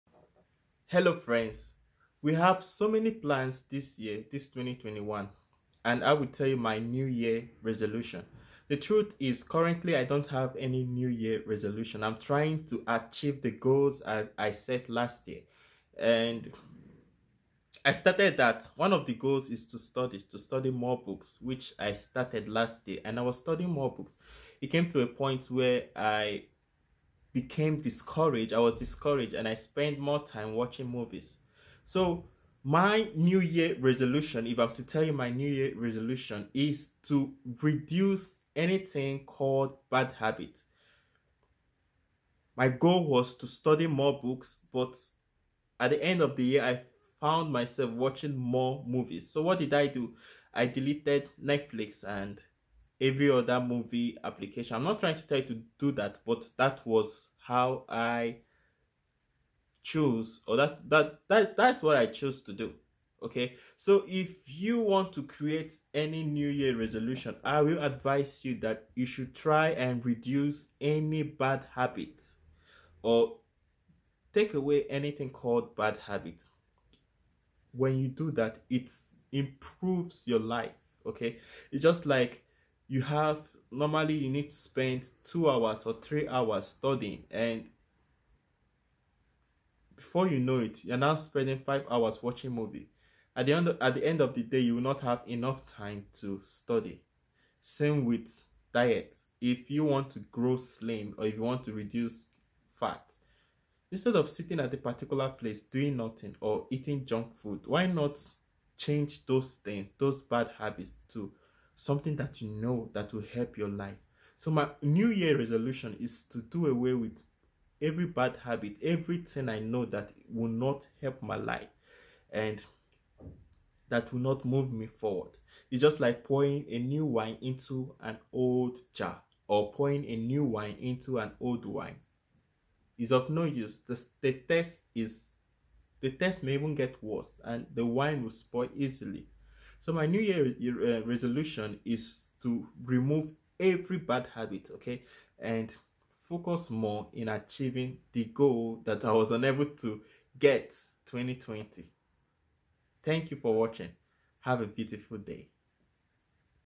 My new year resolution
My new year resolution is avoid every negativity in life
A New Year's resolution is a tradition, most common in the Western Hemisphere but also found in the Eastern Hemisphere, in which a person resolves to continue good practices, change an undesired trait or behavior, accomplish a personal goal, or otherwise improve their life at the start of a new year. Focus on removing the negativities